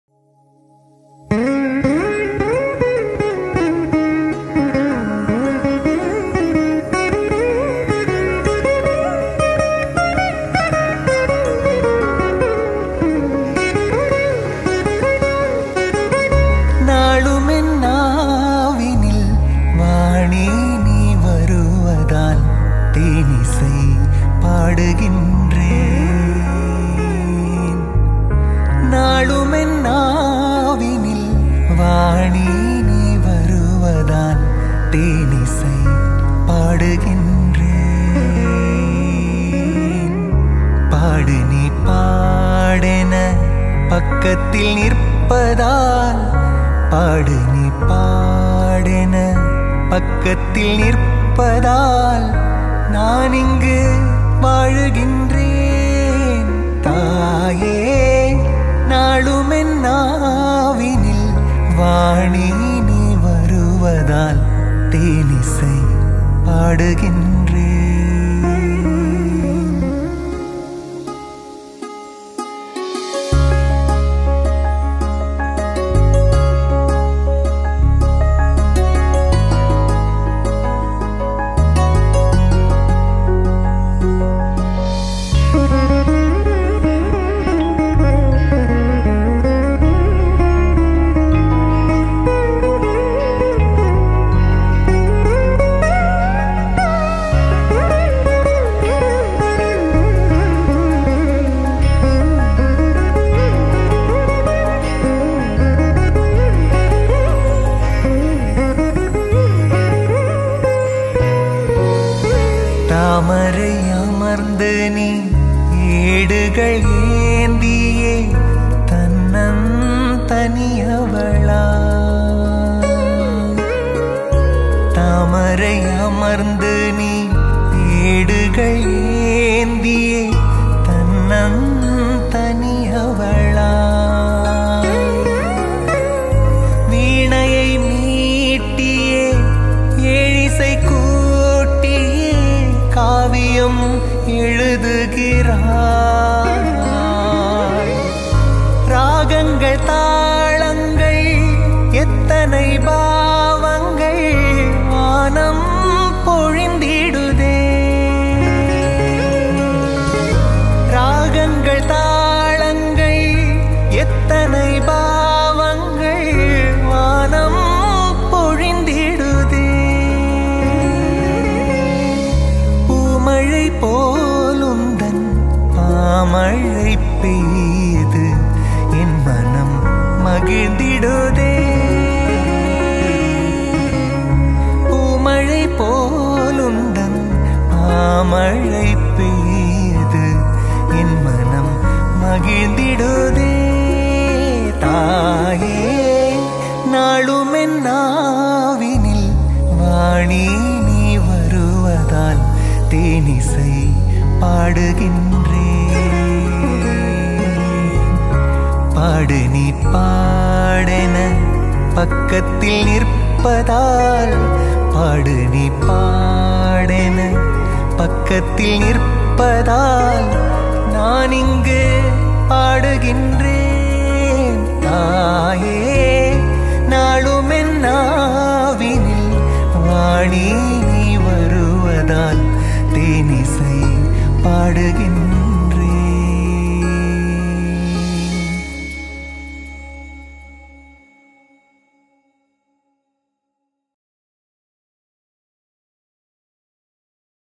Navaratri |Saraswathi |tamil | devotional |punya Srinivas | kaushik menon ft
VOCAL  : Kaushik Menon
VEENA  : Punya Srinivas
Music Programmed by : Alan
Mixed & Mastered by Maniratnam at Tapas studio
Camera :  S.Shanmuga Sundaram (Assisted by S.Magesh )
Editing : Sound Parti Studios

Special thanks to Arjun Muralidharan , Shenbaga Raj & Abihnaya